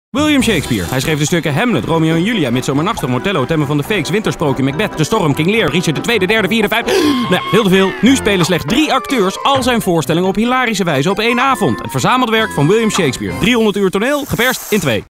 Het verzamelde werk van William Shakespeare HNT
Het verzamelde werk van William Shakespeare
Het Nationale Toneel

19 Oktober in de Stadsgehoorzaal!

Met Jappe Claes, Vincent Linthorst en Bram Suijker
Regie: Theu Boermans

We zijn trots dat we zulke mooie toneelgezelschappen in huis hebben. Neem nu deze voorstelling: 37 Shakespeare stukken ineen. Een razende komedie waarin koningsdrama's verworden tot voetbaltoernooien, alle komedies op één hoop worden gegooid, alle vrouwen vermomde mannen zijn en heel veel sonnetten langskomen.

Een hilarisch huzarenstukje!